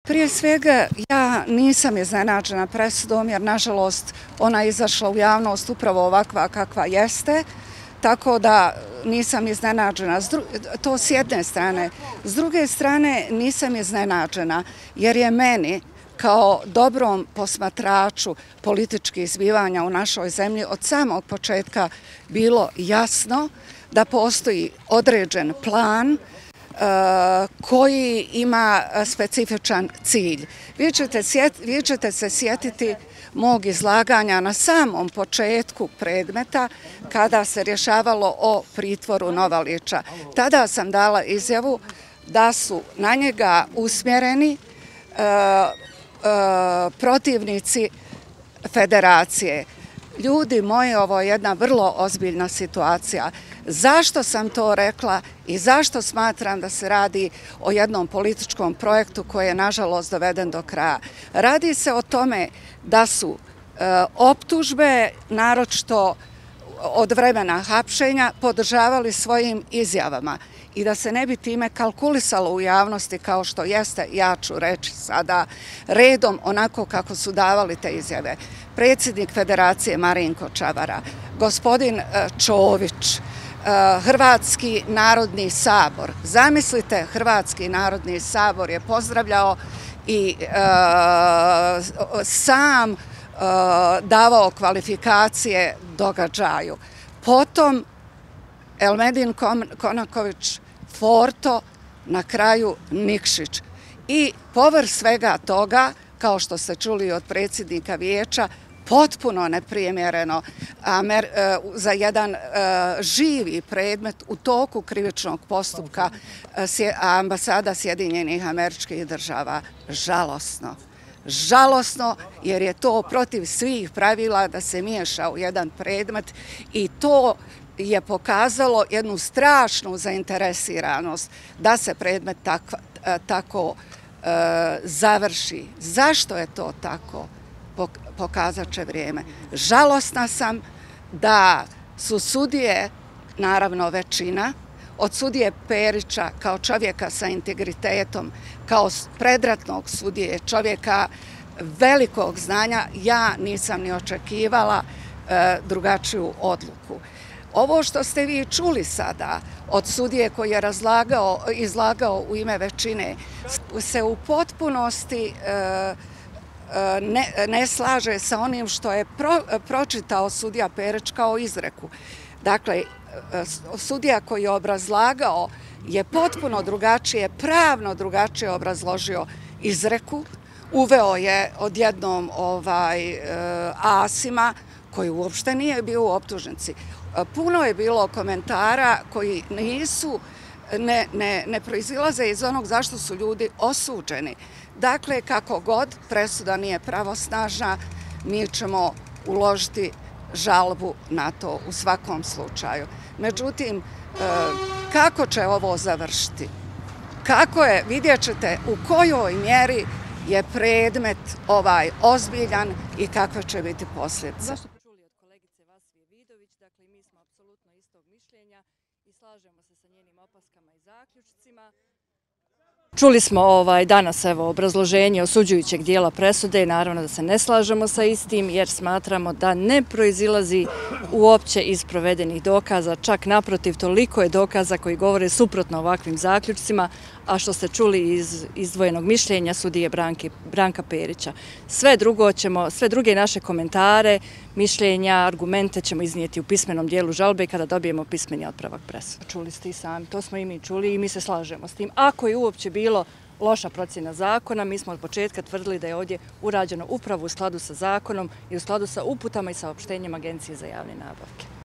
Novalić dobio četiri godine zatvora, advokatica: Ljudi moji, ovo je vrlo ozbiljna situacija
Braniteljica Fadila Novalića, prvostepeno osuđenog u predmetu "Respiratori", Vasvija Vidović kazala je da nije iznenađena presudom jer je ona u javnost izašla upravo onako kako ju je danas saopćilo Sudsko vijeće, te da joj je, kao dobrom poznavaocu političkih prilika u Bosni i Hercegovini, od samog početka bilo jasno da postoji određen plan koji ima specifičan cilj.

Braniteljica prvostepeno optuženog suspendiranog direktora Federalne uprave civilne zaštite Fahrudina Solaka Marsela Bajramović izjavila je danas  da se ne slaže sa obrazloženjem prvostepene presude u slučaju "Novalić Fadil i dr", jer ne proizilazi iz provedenih dokaza.

Pogledajte njihove kratke izjave nakon izlaska iz sudnice!